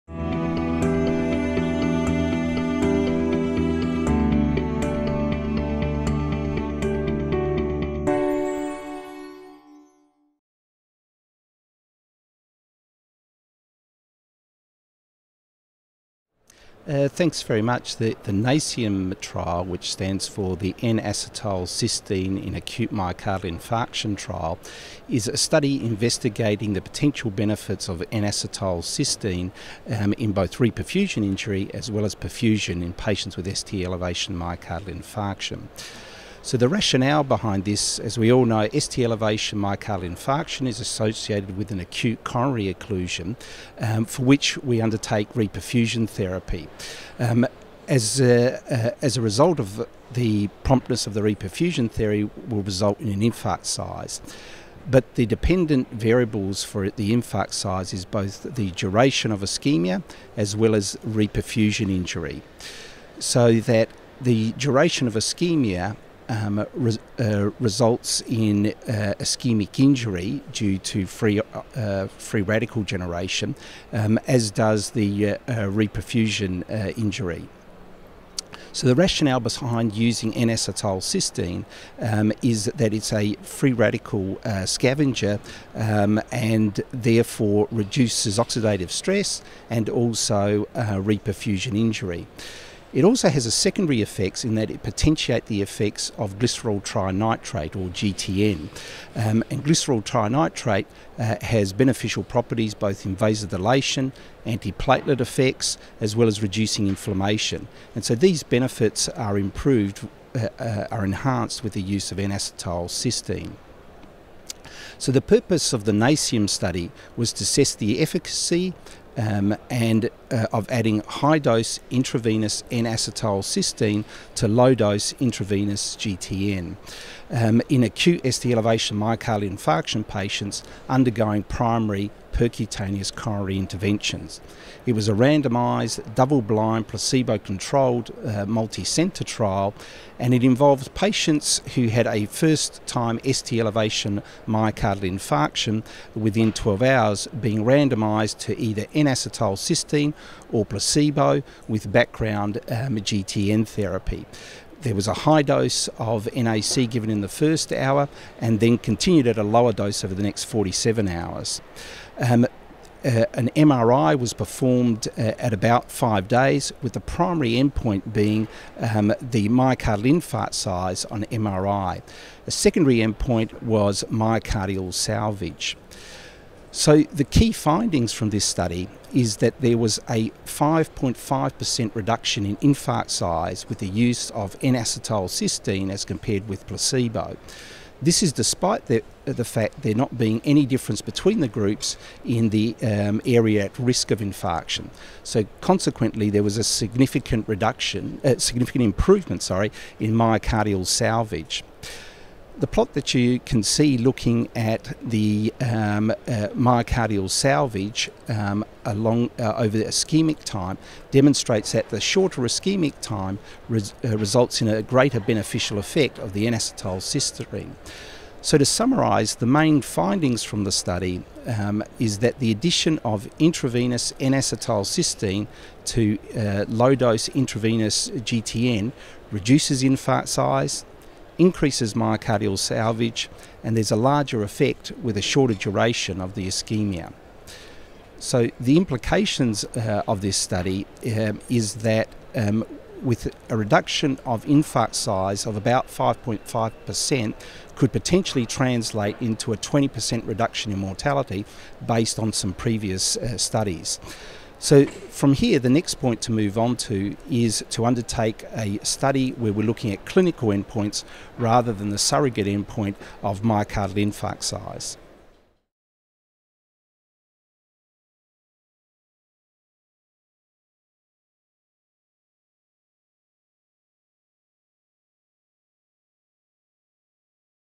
The NACIAM Trial: N-Acetylcysteine reduces infarct size - Prof John Beltrame - ESC2016
Prof John Beltrame from the University of Adelaide, Australia, speaks to Cardio Debate & Radcliffe Cardiology about the NACIAM Trial, during the ESC2016 Congress held in Rome, Italy.

*TRANSCRIPT*
Could you explain the rationale and design behind the NACIAM trial?
The NACIAM trial, which stands for the N-acetylcysteine in Acute Myocardial Infarction trial, is a study investigating the potential benefits of N-acetylcysteine in both reperfusion injury as well as perfusion in patients with ST-elevation myocardial infarction.

The rational behind this is, as we all know ST-elevated myocardial infarction is associated with an acute coronary occlusion, for which we undertake reperfusion therapy. As a result of the promptness of the reperfusion therapy will result in an infarct size. But the dependent variables for the infarct size are the duration of ischaemia as well as reperfusion injury. So the duration of ischemia results in ischaemic injury due to free radical generation, as does the reperfusion injury.

The rational behind using N-acetylcysteine is that it is a free radical scavenger, and therefore reduces oxidative stress and also reperfusion injury. It also has a secondary effect in that it potentiates the effect of glyceryl trinitrate, or GTN.

And GTN has beneficial properties both in vasodilation, anti-platelet effects as well as reducing inflammation. So these benefits are enhanced with the use of N-acetylcysteine.

The purpose of the NACIEM study was to assess the efficacy of adding high-dose intravenous N-acetylcysteine to low-dose intravenous GNT in acute ST-elevation myocardial infarction patients undergoing primary percutaneous coronary interventions.

It was a randomised double-blind placebo-controlled multi-centre trial, and it involved patients who had a first time ST-elevation myocardial infarction within 12 hours of being randomized to either N-acetylcysteine or placebo with background GTN therapy.

It was a high-dose of NACIAN given in the first hour, and then continued at a low dose for the next 47 hours.

An MRI was performed was performed at about five days, the primary end point being the myocardial infarct size on MRI.

The secondary end point was myocardial salvage.

The key findings from this study was that there was a 5.5 per cent reduction in infarct size with the use of N-acetylcysteine compared with placebo. This is despite the fact that there was not any difference between the groups in terms of size of infarction.

Consequently, there was a significant improvement in myocardial salvage. The plot you can see [see video] looking at myocardial salvage demonstrates that the shorter ischaemic time results in a greater beneficial effect of the N-acetylcysteine.

To summarise, the main findings from the study is that the addition of N-acetylcysteine to low-dose intravenous GTN reduces infarct size, increases myocardial salvage and there is a larger effect with a shorter duration of ischaemia.

The implications of this study is that with a reduction in infarct size of about 5.5 per cent could potentially translate in a 20 per cent reduction in mortality based on previous studies.

From here the next point to move on to, is to undertake a study where we’re looking at clinical end points, rather than the surrogate end point of myocardial infarct size.